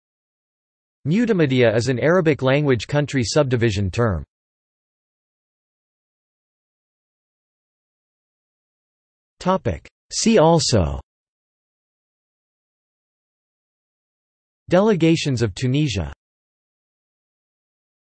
Mutamadiyah | Wikipedia audio article
This is an audio version of the Wikipedia Article:
Mutamadiyah

00:00:05 undefined

- Socrates

SUMMARY
Mutamadiyah is an arabic language country subdivision term.